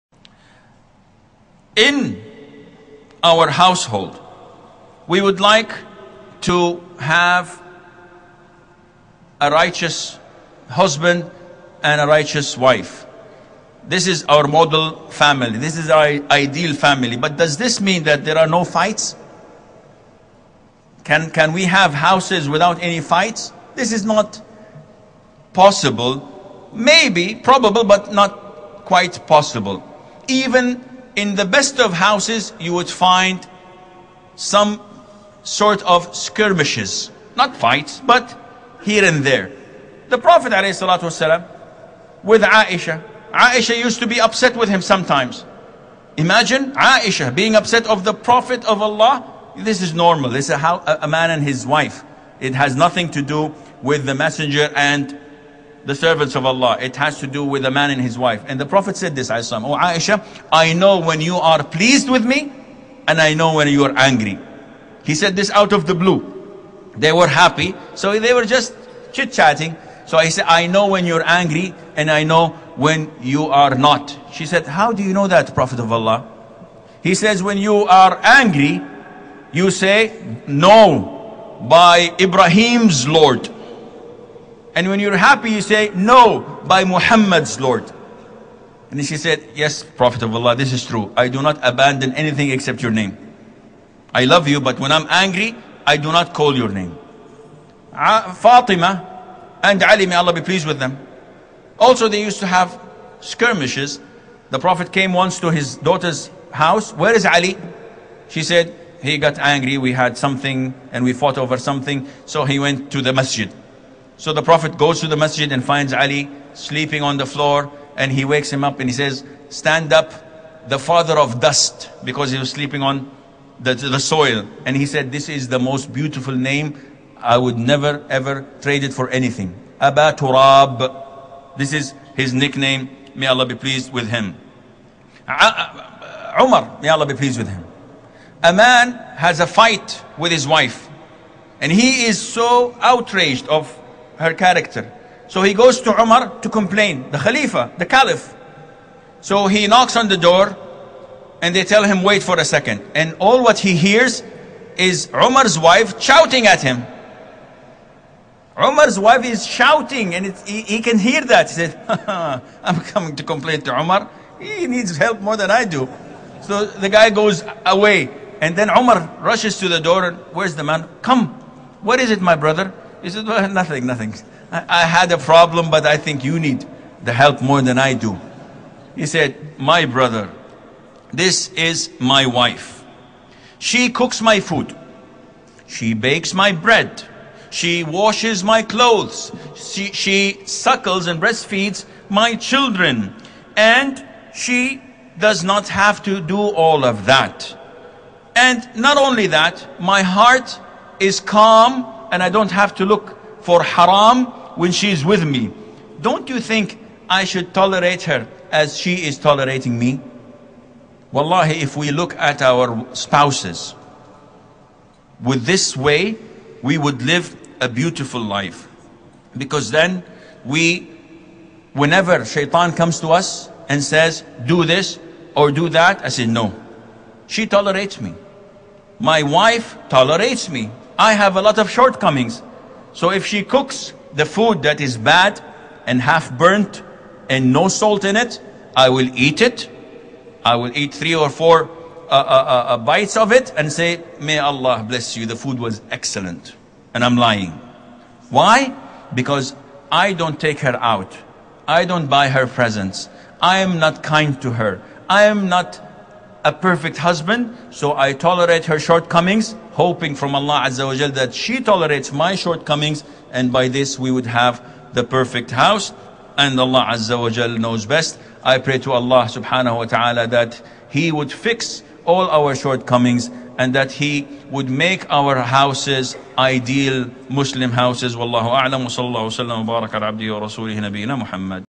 Errors in an Ideal Muslim Couple and How to deal with them-Sheikh Assim Al Hakeem
Clip taken from the lecture "The Model Muslim Family by Sheikh Assim Al-Hakeem"